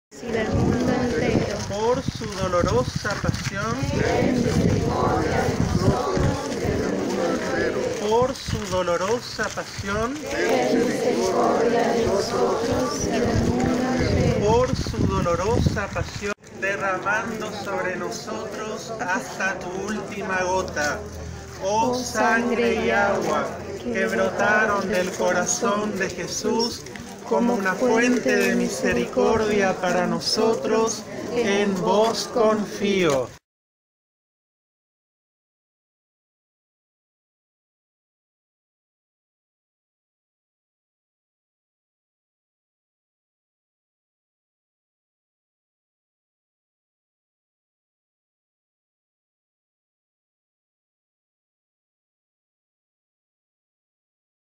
PROCESIÓN DE JESÚS MISERICORDIOSO DOMINGO DE LA DIVINA MISERICORDIA 24 DE ABRIL DE 2022
Procesión de Jesús Misericordioso en el Domingo de la Divina Misericordia (Segundo Domingo de Pascua) realizada por las calles del Barrio Villa Rivadabia de la Ciudad de Mercedes (Provincia de Corrientes) República Argentina por la Parroquia San Ramón Nonato de la misma ciudad, luego de la Celebración de la Santa Misa presidida por el Padre Juan Ramón Lezcano a las 15hs. (LA HORA DE LA GRAN MISERICORDIA DEL SEÑOR PARA EL MUNDO ENTERO).